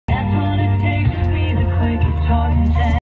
Dodge Charger pulling a trailer, Pretending it's a Ram
Dodge Charger TRUCKING along. Charger pulling a trailer at max load. Who needs a Dodge Ram!